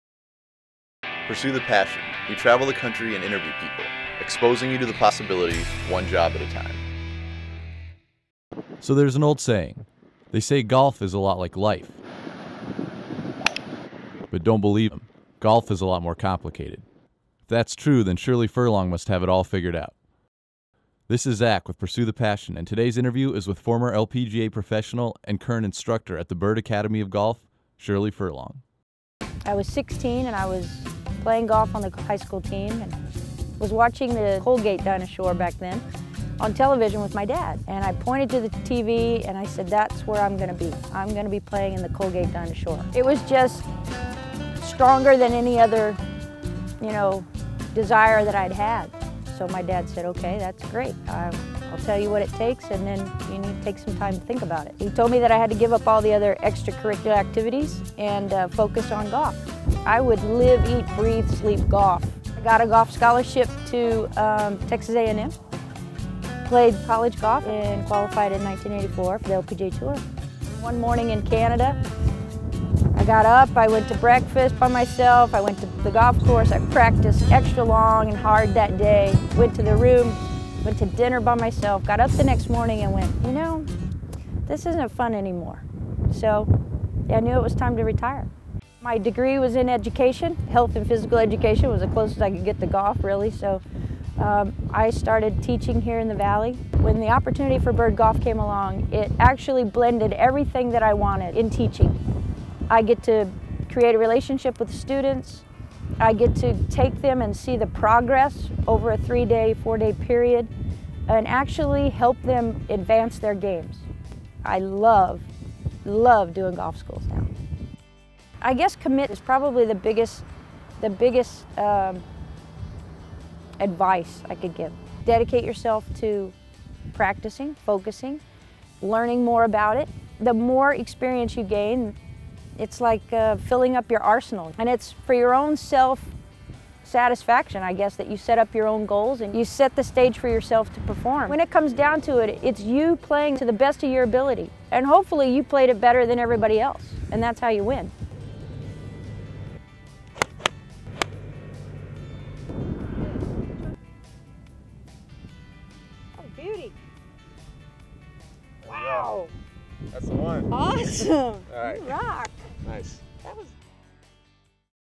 Career Interview- Golf Professional
Career interview with Golf Instructor and former LPGA Professional Shirley Furlong.